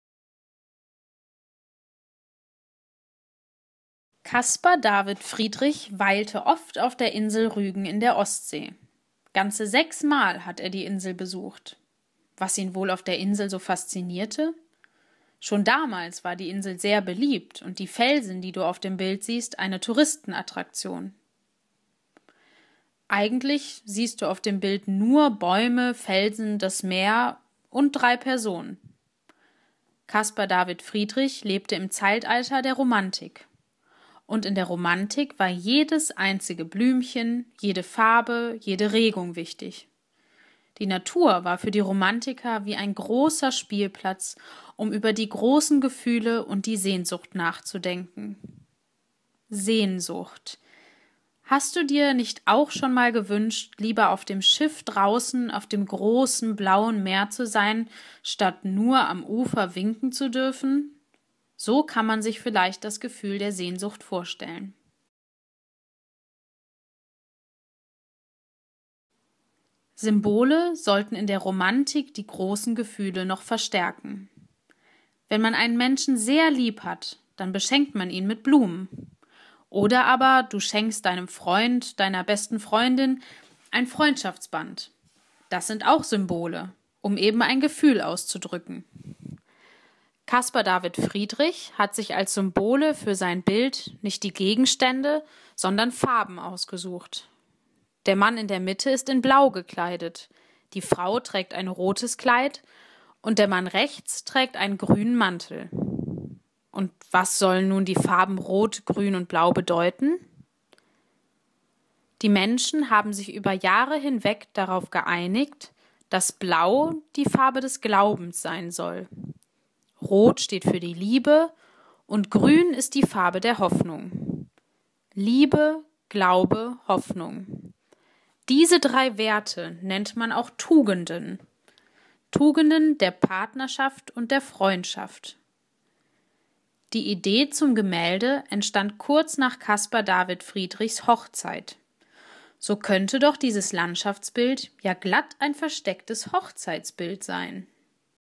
43. Caspar David Friedrich, Kreidefelsen auf Rügen, 1818, Museum Oskar Reinhart, Winterthur, Schweiz
43. Caspar David Friedrich, Kreidefelsen auf Rügen, 1818, Museum Oskar Reinhart, Winterthur, Schweiz

Die Ostseeinsel Rügen
Caspar David Friedrich weilte oft auf der Insel Rügen in der Ostsee. Ganze sechs Mal hat er die Insel besucht!

Was ihn wohl auf der Insel so faszinierte? Schon damals war die Insel sehr beliebt und die Felsen, die Du auf dem Bild siehst, eine Touristenattraktion.

Ein Hochzeitsfoto?
Eigentlich siehst Du auf dem Bild „nur“ Bäume, Felsen, das Meer – und drei Personen.

Caspar David Friedrich lebte im Zeitalter der Romantik! Und in der Romantik war jedes einzige Blümchen, jede Farbe, jede Regung wichtig. Die Natur war für die Romantiker wie ein großer Spielplatz, um über die großen Gefühle und die Sehnsucht nachzudenken.

Sehnsucht….. Hast Du Dir nicht auch schon mal gewünscht, lieber auf dem Schiff draußen auf dem großen, blauen Meer zu sein statt „nur“ am Ufer winken zu dürfen….? So kann man sich vielleicht das Gefühl der Sehnsucht vorstellen.

Symbole
Symbole sollten in der Romantik die großen Gefühle noch verstärken. Wenn man einen Menschen sehr lieb hat, dann beschenkt man ihn mit Blumen. Oder aber Du schenkst Deinem Freund/Deiner besten Freundin ein Freundschaftsband. Das sind auch Symbole, um eben ein Gefühl auszudrücken.

Caspar David Friedrich hat sich als Symbole für sein Bild nicht Gegenstände, sondern Farben ausgesucht: Der Mann in der Mitte ist in Blau gekleidet. Die Frau trägt ein rotes Kleid. Und der Mann rechts trägt einen grünen Mantel. Und was sollen nun die Farben rot, grün und blau bedeuten?

Die Menschen haben sich über Jahre hinweg darauf geeinigt, dass blau die Farbe des Glaubens sein soll. Rot steht für die Liebe. Und grün ist die Farbe der Hoffnung.

Liebe, Glaube, Hoffnung – diese drei Werte nennt man auch Tugenden. Tugenden der Partnerschaft und der Freundschaft.

Die Idee zum Gemälde entstand kurz nach Caspar David Friedrichs Hochzeit. So könnte doch dieses Landschaftsbild ja glatt ein verstecktes „Hochzeitsbild“ sein….?